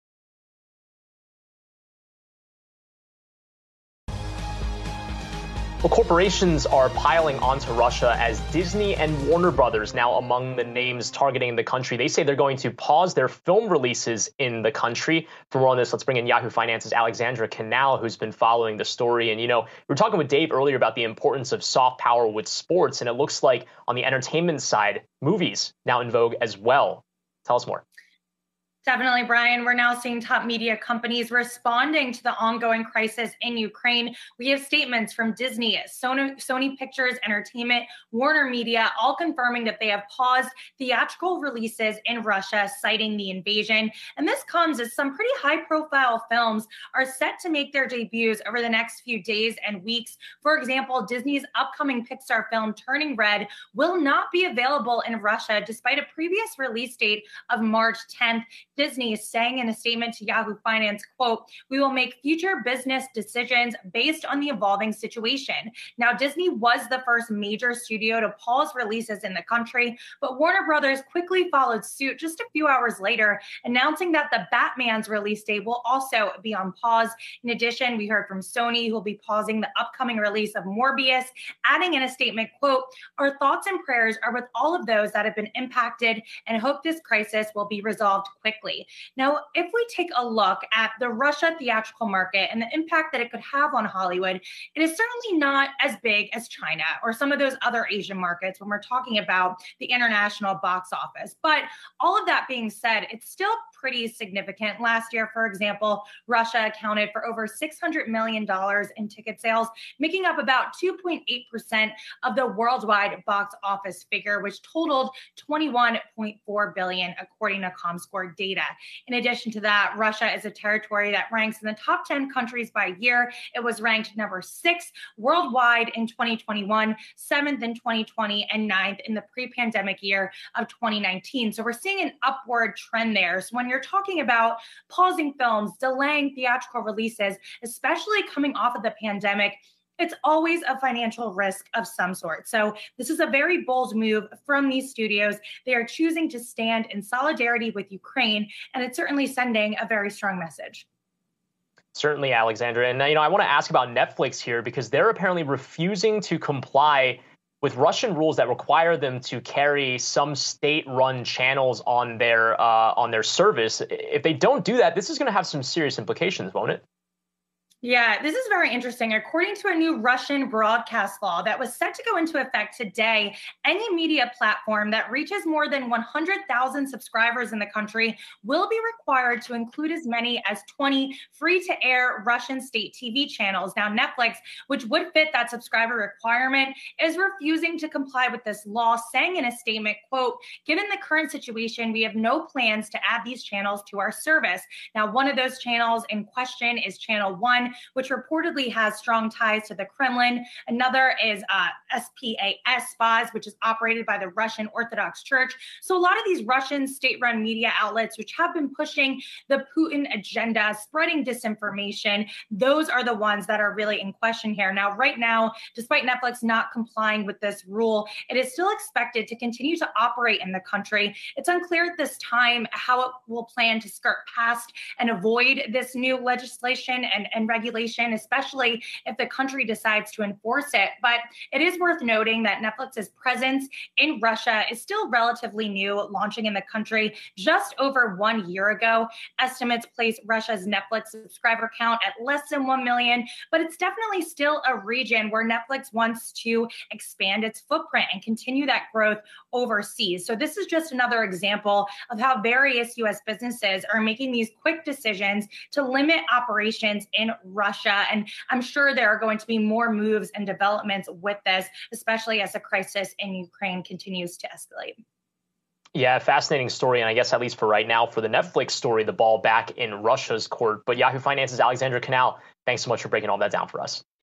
Disney, Sony, Warner Media pause movie releases in Russia
Yahoo Finance's Alexandra Canal reports on Disney, Warner Media, and Sony pausing all upcoming movie releases in Russia amid the Russia-Ukraine war while Netflix has cut Russian channels from its service.